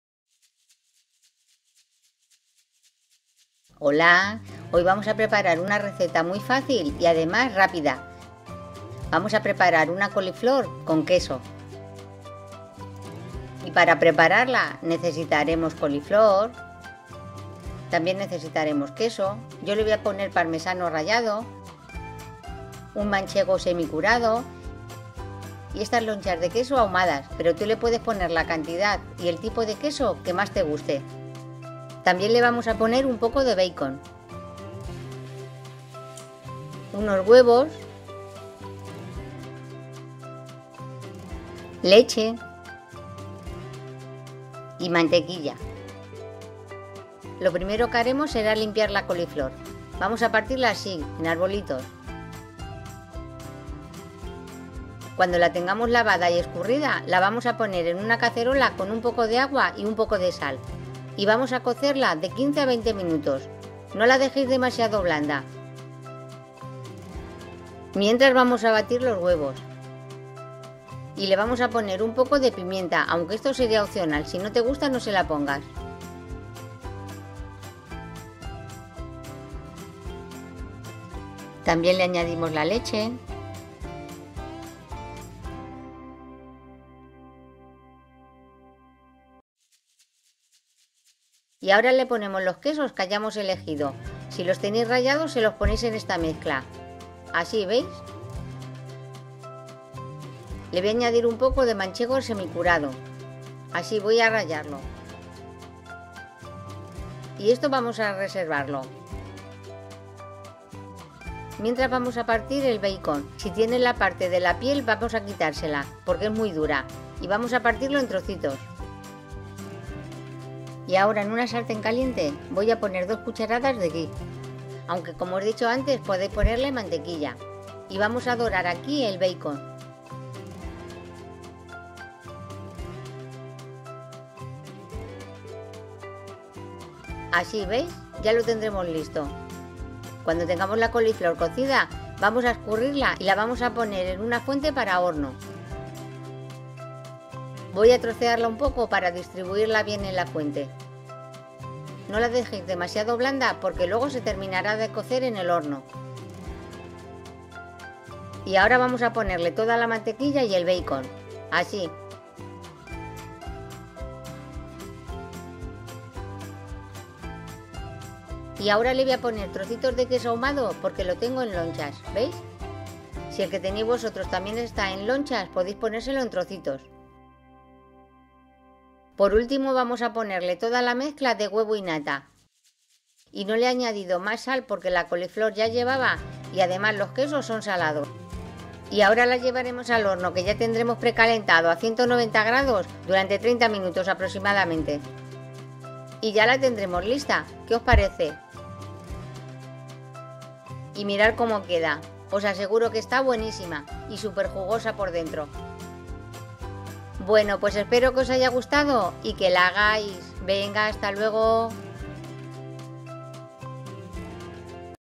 ME ENCANTA ESTA RECETA!!! (COLIFLOR GRATINADA CON QUESO)
ESTA RECETA TE VA A ENCANTAR, SIMPLEMENTE PORQUE ES MUY FÁCIL Y RÁPIDA DE HACER Y ADEMÁS ESTÁ BUENÍSIMA.

INGREDIENTES:

- 1 coliflor
- 50gr de beicon
- mezcla de quesos
. 3 huevos
- 1L. de leche o 500ml de nata (crema de leche)
- 2cud de mantequilla

VISITA MI CANAL SECUNDARIO:
YOKETO RECETAS